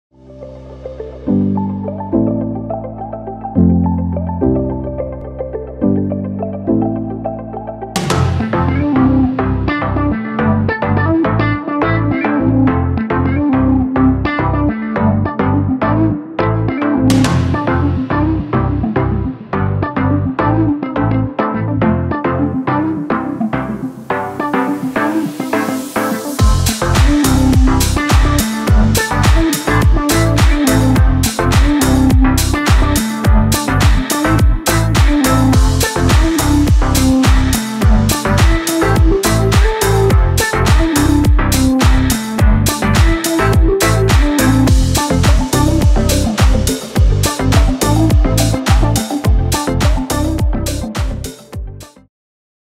Enders FANCY // Terrassenheizer Gas // ECO-Plus Brenner 8kW
Wenn es kühl wird, ist der Fancy genau der Terrassenheizer, der Deine Party auf Temperatur hält. Sein elegantes Auftreten, hochwertige Materialien und vor allem der praktische Ablagetisch aus Kunststoff laden zum Verweilen ein. Unter dem Wärmeeinfluss des effektiven 8 kW-Brenners können Deine Partygäste richtig Gas geben, ohne dass der Terrassenheizer viel davon verbraucht. Übrigens auch nicht beim Platz: durch die versenkbare Säule kann der Fancy anschließend platzsparend verstaut werden.